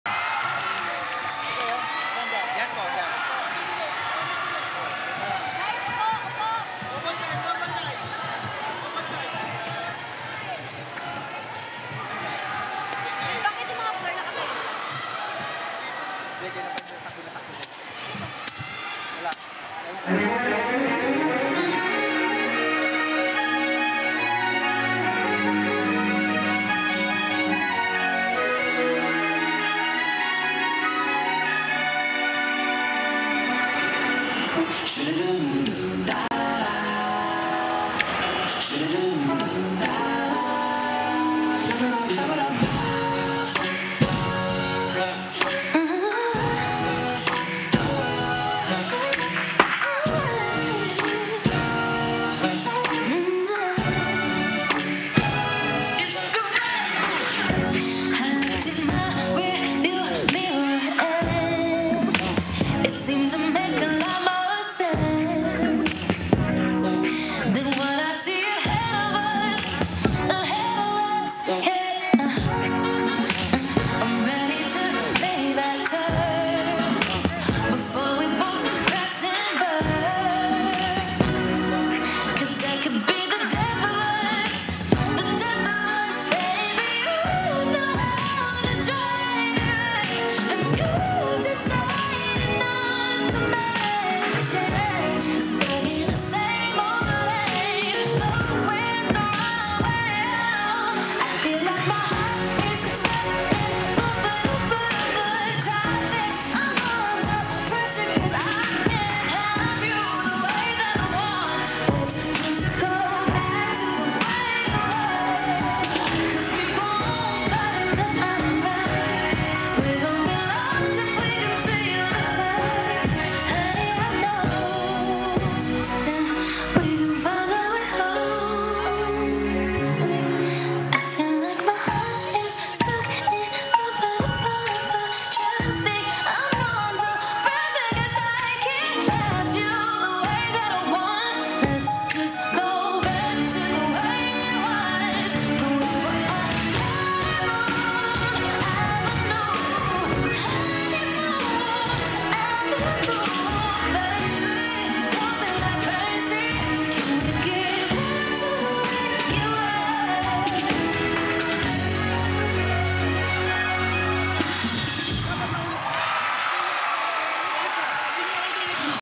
2024 NDC SAGHH - SVNHS D'Squad (Signal Village National HS)
2024 NDC at Philsports Arena saw over 70 teams compete